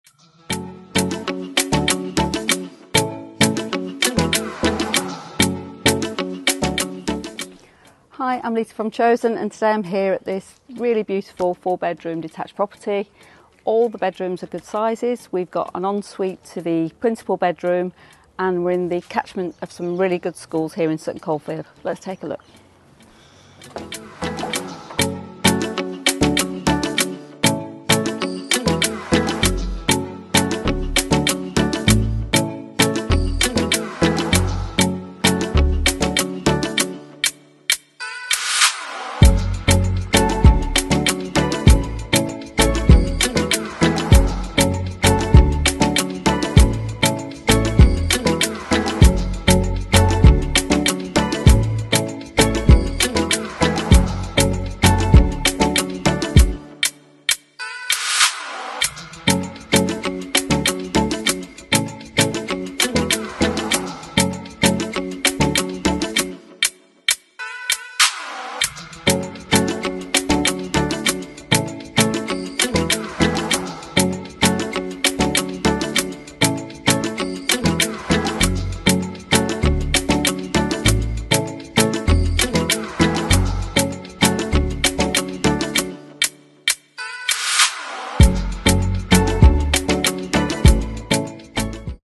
Wheatmoor Road, Sutton Coldfield | Chosen Home | Property Tour 🏡
Welcome to Wheatmoor Road…
This stunning property located on Wheatmoor Road in the sought-after area of Sutton Coldfield boasts 3 reception rooms, and a modern kitchen/diner providing ample space for entertaining guests or simply relaxing with your loved ones. With 4 double bedrooms, there is plenty of room for a growing family or for those who enjoy having a home office or guest room.